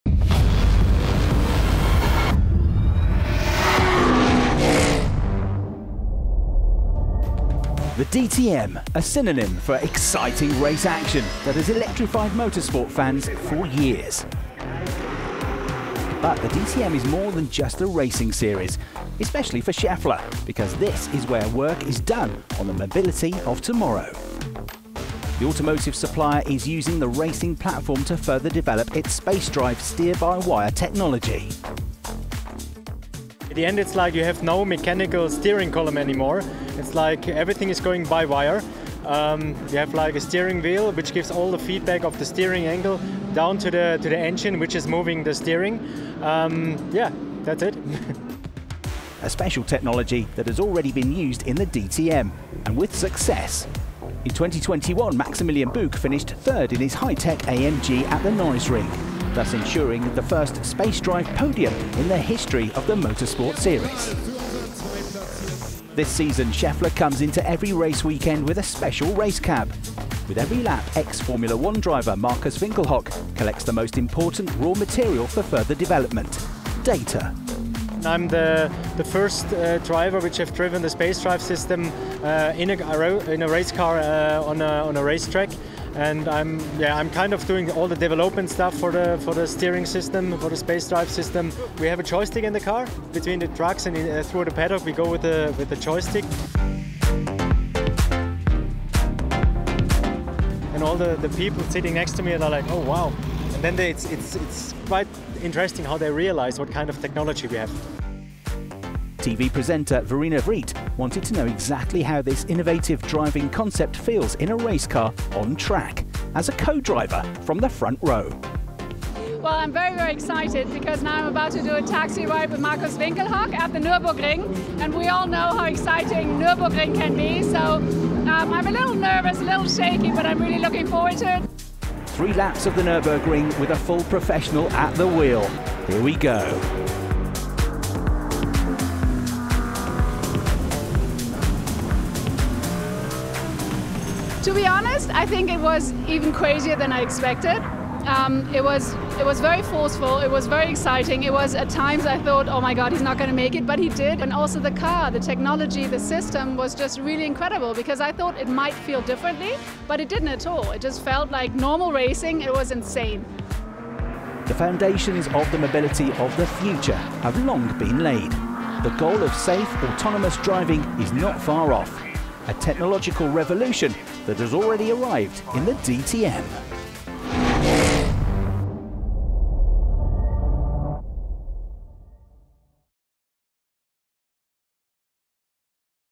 DTM Innovation Taxi with Space Drive
Our Audi R8 LMS GT2 Innovation Taxi demonstrates impressively in DTM what the Steer-by-Wire steering system Space Drive is capable of. It can even be steered solely using a joystick. Racing driver Markus Winkelhock explains how this works and what it feels like in the technology feature.